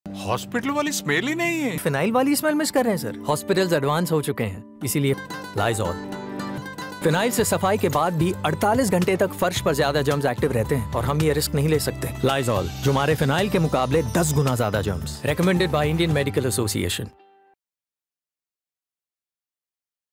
Lizol HCP TVC "Healthy home starts with Lizol!"
Did you know illness-causing germs are active on your floor for 48 hours after cleaning with phenyl?
Recommended by the Indian Medical Association, Lizol Floor Cleaner kills 10x more germs than phenyl, keeping your home healthy and your loved ones safe. Switch from phenyl today.